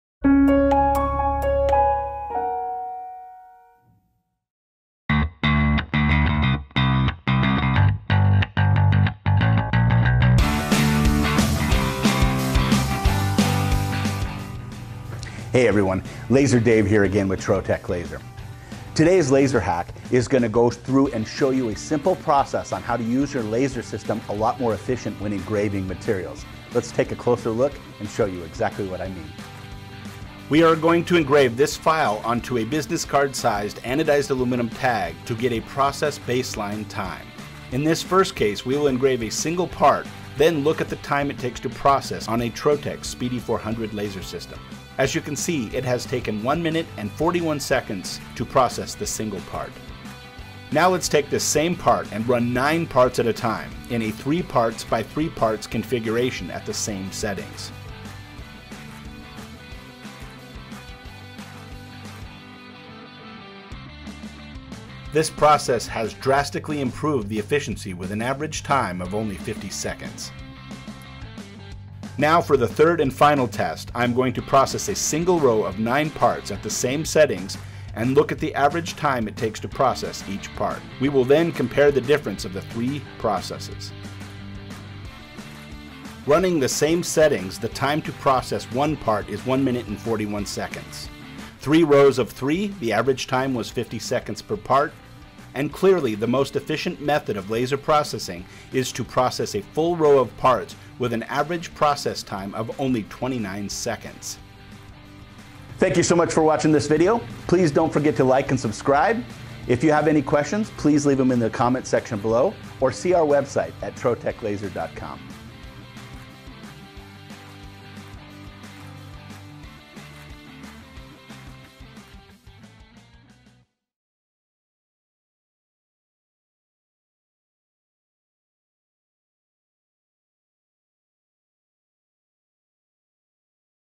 Laser Hack: Engraving Single vs Row of Parts
Which is faster: engraving a single item or an entire row at once? Laser Dave explains how engraving a row of items can cut down the processing time of an individual part in this week's

Want even more information on laser engraving and cutting?

Be the first to get updates!